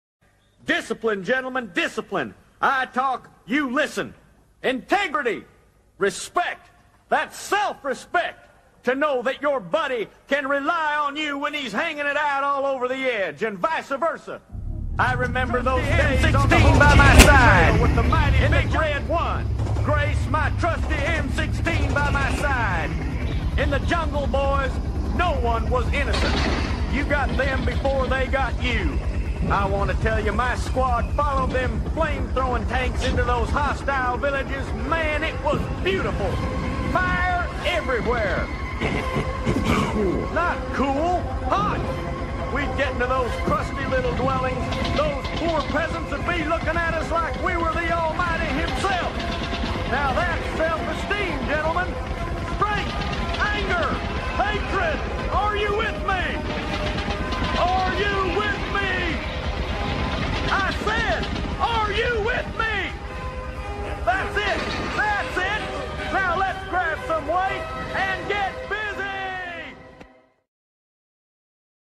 1996 Desert Storm(2nd Gulf War) ARNG 2nd Infantry- Long Range Recon Patrol
Staff sergeant E-6 Jonathan Jimmy " Snake " Shearman, Operation Tiger 1, Baker team, 1996 Desert Storm, NBC guerrilla wars, Africa&Iraq, Army Reserve 28th Infantry Special Forces troops division ghost reconaissance and night operations.